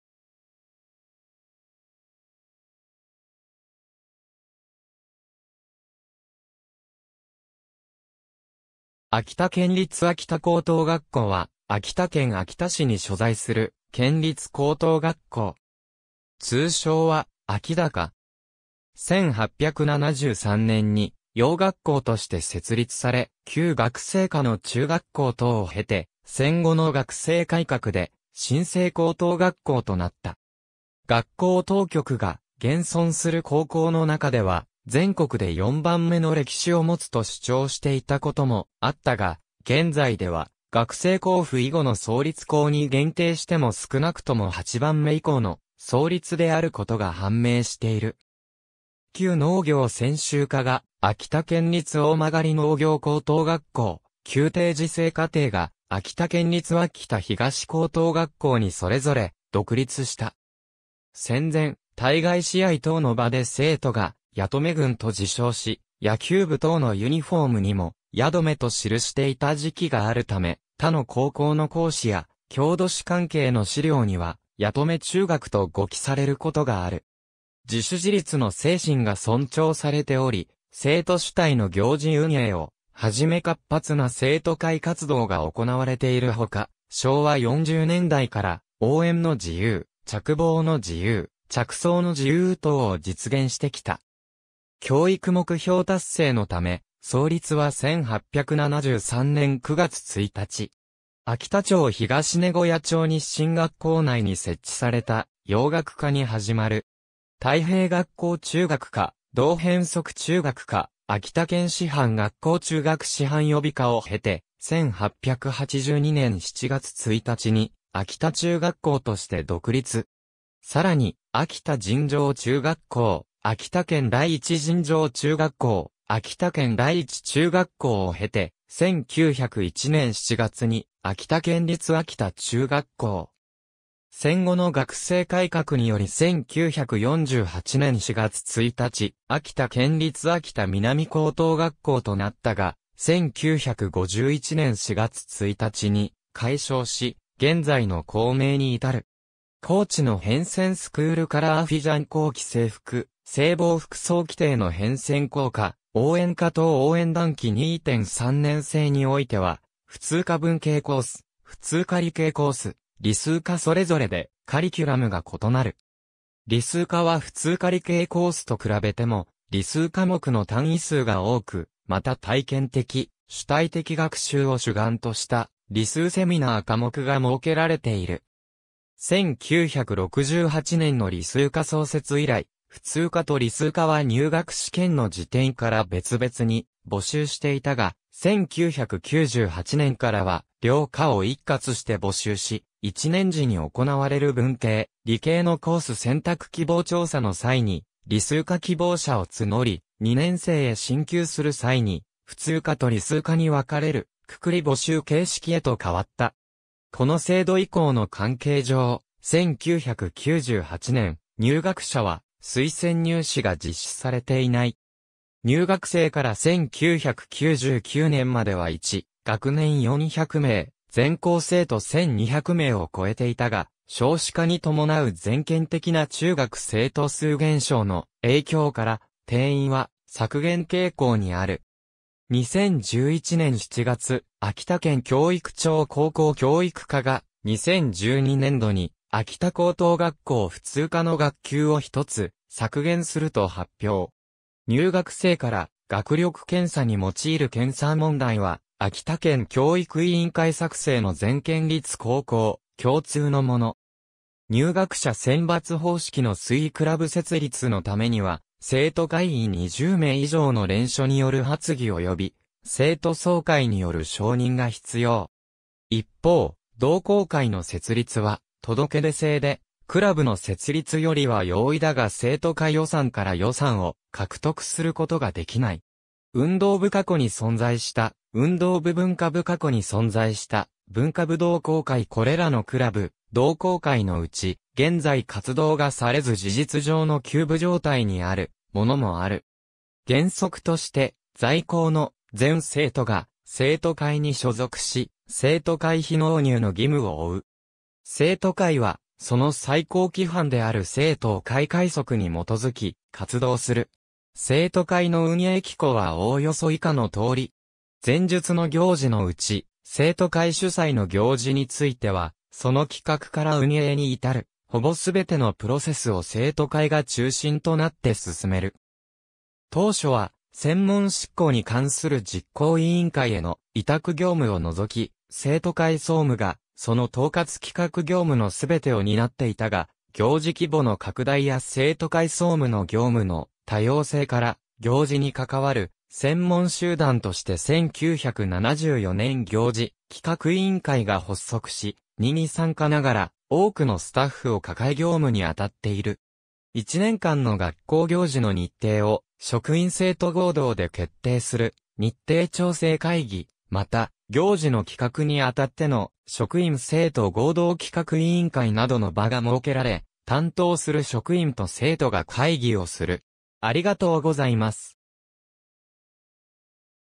秋田県立秋田高等学校
秋田県立秋田高等学校（あきたけんりつあきたこうとうがっこう）は、秋田県秋田市に所在する県立高等学校。
通称は「秋高」（しゅうこう）。
1873年（明治6年）に洋学校として設立され、旧学制下の中学校等を経て、戦後の学制改革で新制高等学校となった。
学校当局が「現存する高校の中では全国で4番目の歴史を持つ」と主張していたこともあった が、現在では学制公布以後の創立校に限定しても少なくとも8番目以降の創立であることが判明している。
旧農業専修科が秋田県立大曲農業高等学校、旧定時制課程が秋田県立秋田東高等学校にそれぞれ独立した（年表を参照）。
戦前、対外試合等の場で生徒が「矢留（やどめ）軍」と自称し、野球部等のユニフォームにも「YADOME」と記していた時期があるため、他の高校の校史や郷土史関係の資料には「矢留中学」と誤記されることがある。
自主自律の精神が尊重されており、生徒主体の行事運営をはじめ活発な生徒会活動が行われている ほか、昭和40年代から「応援の自由」「着帽の自由」「着装の自由」 等を実現してきた。
教育目標達成のため、 創立は1873年（明治6年）9月1日。
秋田町東根小屋町日新学校内に設置された洋学科（洋学校と称する）に始まる。
太平学校中学科、同変則中学科、秋田県師範学校中学師範予備科を経て、1882年（明治15年）7月1日に秋田中学校として独立。
さらに秋田尋常中学校、秋田県第一尋常中学校、秋田県第一中学校を経て、1901年（明治34年）7月に秋田県立秋田中学校。
戦後の学制改革により1948年（昭和23年）4月1日、秋田県立秋田南高等学校となったが、1951年（昭和28年）4月1日に改称し、現在の校名に至る。
校地の変遷(folding) スクールカラー 徽章 校旗 制服・制帽 服装規定の変遷(folding) 校歌・応援歌等 応援団旗 2・3年生においては、普通科文系コース、普通科理系コース、理数科それぞれでカリキュラムが異なる。
理数科は普通科理系コースと比べても理数科目の単位数が多く、また体験的・主体的学習を主眼とした「理数セミナー」科目が設けられている。
1968年（昭和43年度）の理数科創設以来、普通科と理数科は入学試験の時点から別々に募集していたが、1998年（平成10年度）からは両科を一括して募集し、1年時に行われる文系・理系のコース選択希望調査の際に理数科希望者を募り、2年生へ進級する際に普通科と理数科に分かれる「くくり募集」形式へと変わった。
この制度移行の関係上、1998年（平成10年度）入学者は推薦入試が実施されていない（推薦入試は翌年から復活し、現在は後述の前期選抜の形式をとっている）。
（2006年（平成18年度）入学生から） 1999年（平成11年度）までは一学年400名、全校生徒1200名を超えていたが、少子化に伴う全県的な中学生徒数減少の影響から、定員は削減傾向にある。
2011年（平成23年）7月、秋田県教育庁高校教育課が、2012年度に秋田高等学校普通科の学級を1つ削減すると発表。
（2005年（平成17年度）入学生から） 学力検査に用いる検査問題は秋田県教育委員会作成の全県立高校共通のもの。
入学者選抜方式の推移(folding) クラブ（運動部または文化部）設立のためには生徒会員20名以上の連署による発議及び生徒総会による承認が必要。
一方、同好会の設立は届出制で、クラブの設立よりは容易だが生徒会予算から予算を獲得することができない。
運動部 過去に存在した運動部(folding) 文化部 過去に存在した文化部(folding) 同好会 これらのクラブ・同好会のうち、現在活動がされず事実上の休部状態にあるものもある。
原則として在校の全生徒が生徒会 に所属し、生徒会費納入の義務を負う。
生徒会はその最高規範である生徒お会会則 に基づき活動する。
生徒会の運営機構はおおよそ以下のとおり。
前述の行事のうち、生徒会主催の行事についてはその企画から運営に至るほぼすべてのプロセスを生徒会（特に行事企画委員会）が中心となって進める。
当初は専門執行に関する実行委員会への委託業務を除き、生徒会総務がその統括企画業務のすべてを担っていたが...